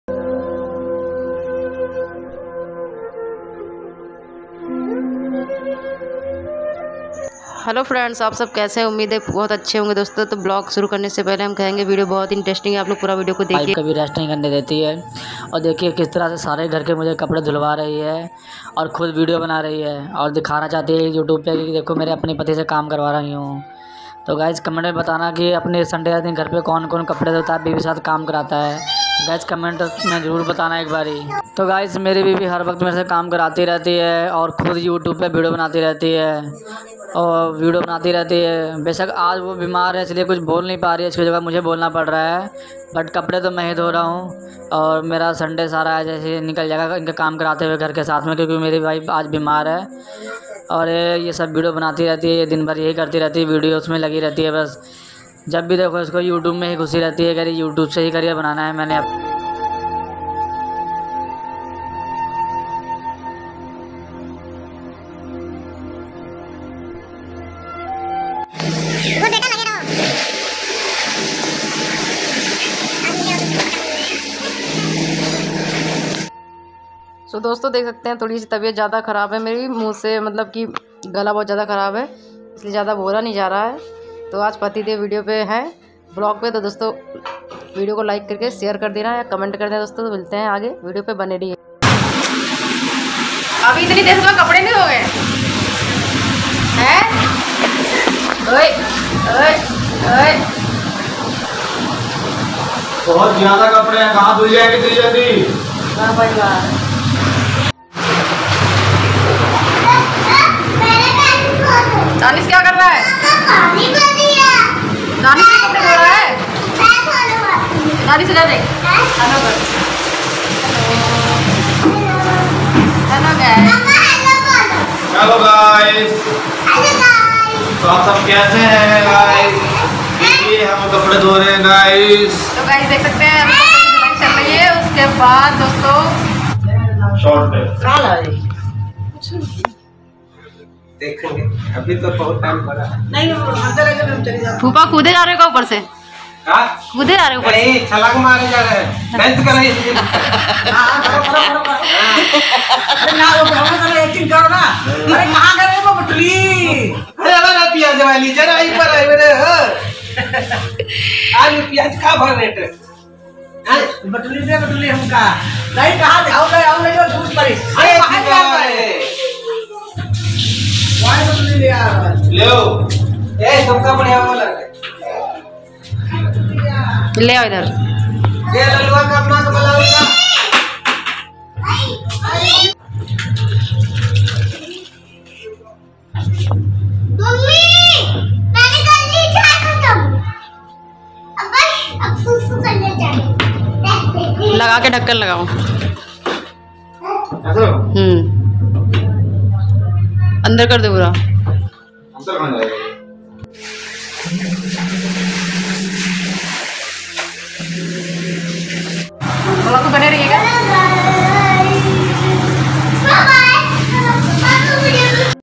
मेरी बीवी मेरे से बहुत काम कराती है 😭||Mere Pati ne Aaj sare kapde Ghar Ke 😂😂artivlogers 🙏🙏
मेरी बीवी मेरे से बहुत काम कराती है 😭||Mere Pati ne Aaj sare kapde Ghar Ke dhoye 😂😂artivlogers 🙏🙏